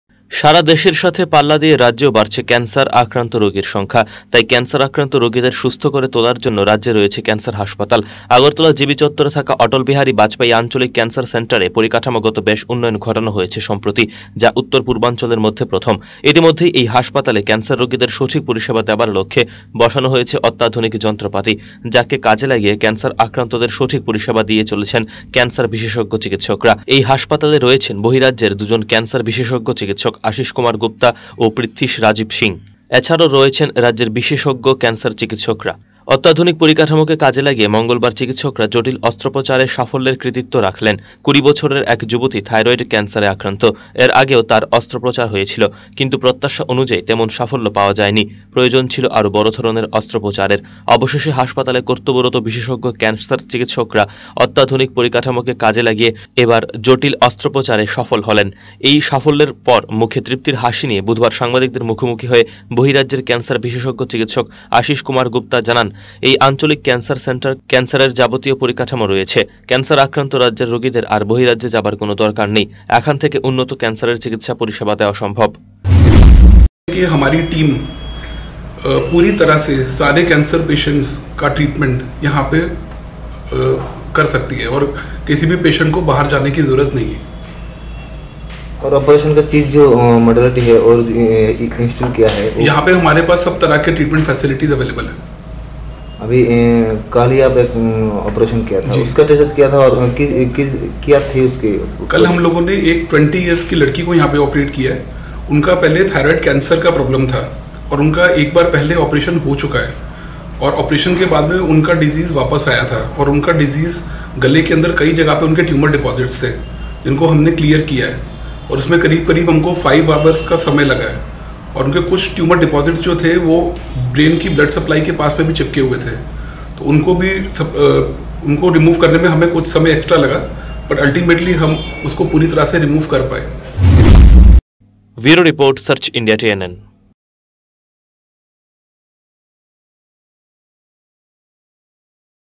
অত্যাধুনিক পরিকাঠামো
রাজ্যের ক্যান্সার রোগীদের অন্যতম চিকিৎসা কেন্দ্র অটল বিহারি বাজপেয়ি আঞ্চলিক ক্যান্সার সেন্টারে গড়ে উঠেছে অত্যাধুনিক পরিকাঠামো। ক্যান্সার আক্রান্ত রোগীদের জটিল অস্ত্রপচারে এবার রাজ্যের নজির স্থাপন করল এই হাসপাতাল। সেন্টারের ক্যান্সার সার্জনদের দাবি, এখানে ক্যান্সার এর যাবতীয় পরিকাঠামো রয়েছে।